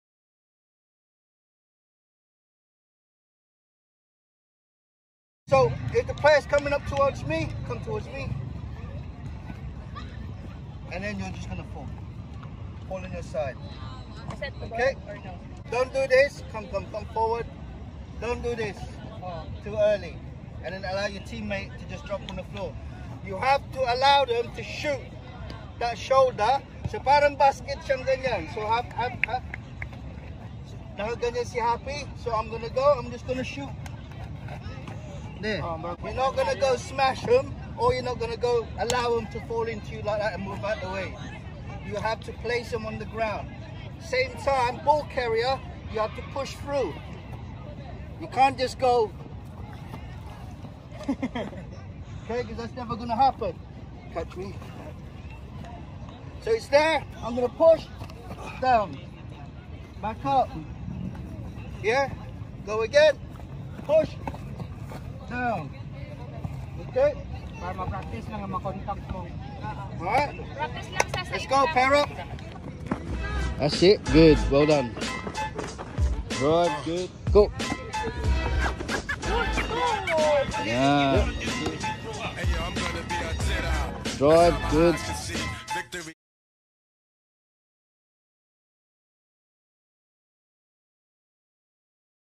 Episode 1.0 Pre Tackle Drills to help introduce contact rugby.
Introducing contact rugby to the locals of Cagayan de Oro City. We don’t often get to train with tackle pads so Coach Noel Villa is teaching the “newbies” how to safely land on the ground on their side when making contact from a tackle. Although this isn’t a tackle drill, it’s important to remember most of these guys/gals have never played rugby before and it’s the safest way for them to learn to hit the ground with the “meaty” part of their shoulder.
Remember 3🔑 things.
•• Power Drive
••• Fall on your side

For Progression
1• Use FOOTWORK just before contact
2• Tempo or Speed of the drill. Make it more game like speed but keeping the Impact at 80% for safety
3• Ball Carrier upon landing will place ball towards team mates and have another player in support to form a RUCK.